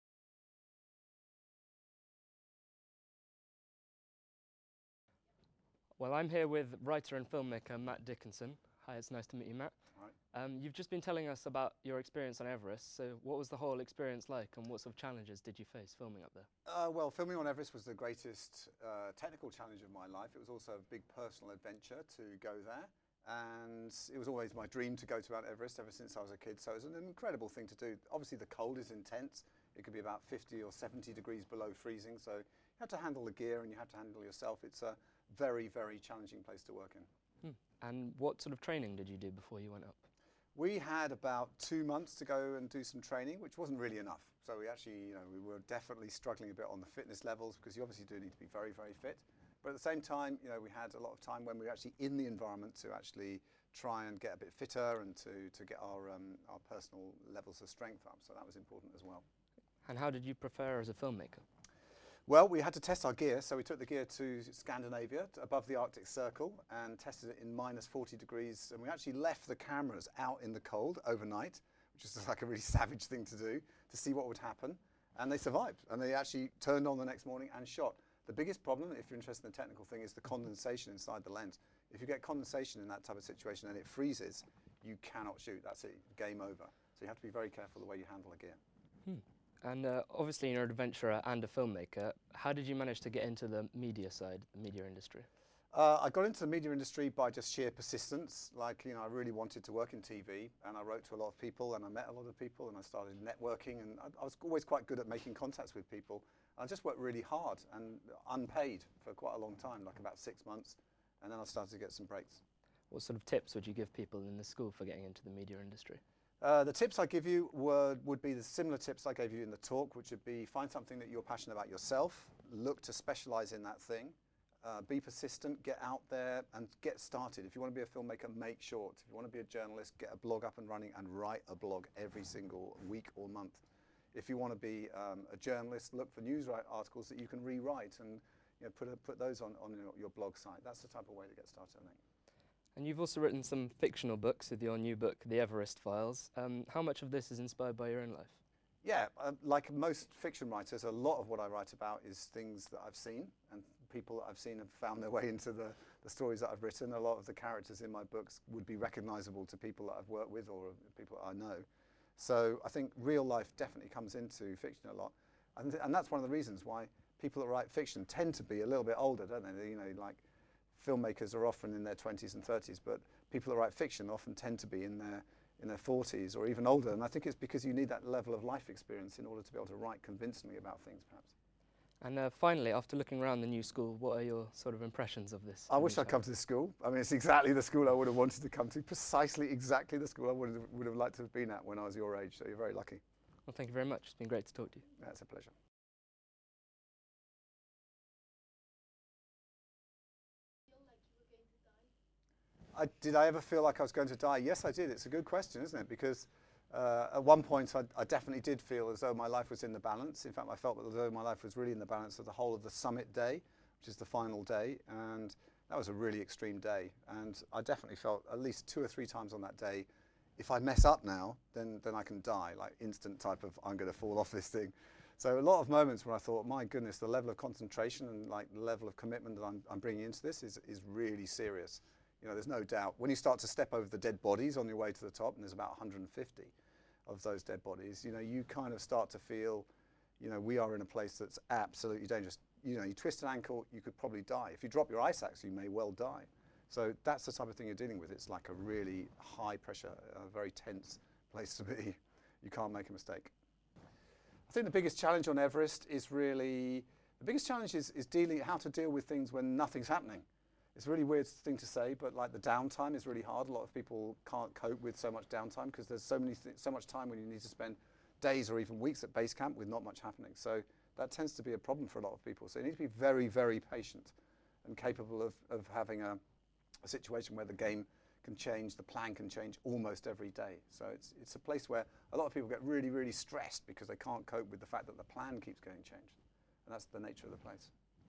Matt Dickinson Interview at The Bath Studio School
Fraser Stone interviews Matt Dickinson about his experiences of Mt Everest and his new book The Everest Files